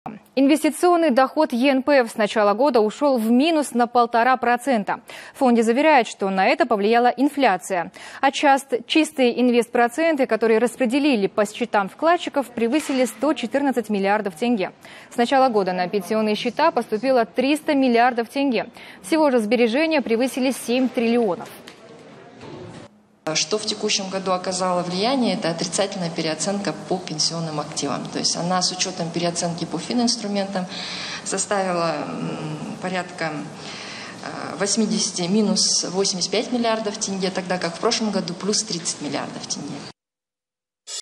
Инвестиционный доход ЕНПФ в Казахстане с начала года ушел в минус (27.06.17)
Инвестиционный доход ЕНПФ с начала года ушел в минус на полтора процента. В фонде заверяют, что на это повлияла инфляция. А чистые инвест проценты, которые распределили по счетам вкладчиков, превысили 114 миллиардов тенге. С начала года на пенсионные счета поступило 300 миллиардов тенге. Всего же сбережения превысили 7 триллионов.